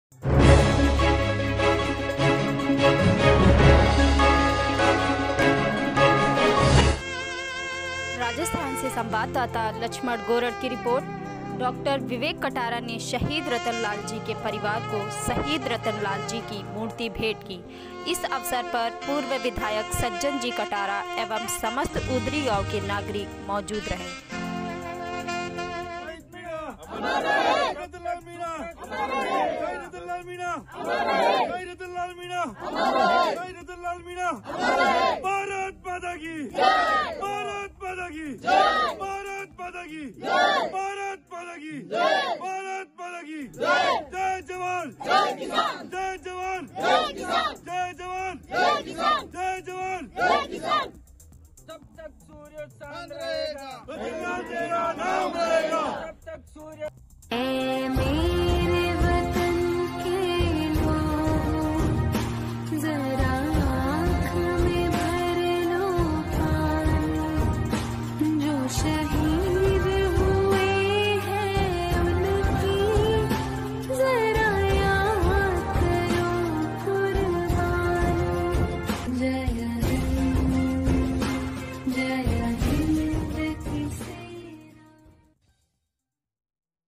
# maa Anandi news राजस्थान से संवाददाता लक्ष्मण गोरण रिपोर्टआज दिनांक 26/11/2020को डॉ. विवेक कटारा
राजस्थान से संवाददाता लक्ष्मण गोरण रिपोर्टआज  दिनांक 26/11/2020को  डॉ. विवेक कटारा ने शहीद रतन लाल जी के परिवार को शहीद रतन लाल जी की मूर्ति भेट करी इस अवसर पर पूर्व विधायक सज्जन जी कटारा एवं समस्त  ऊंदरी गाँव के नागरिक रहे मौजूद  ।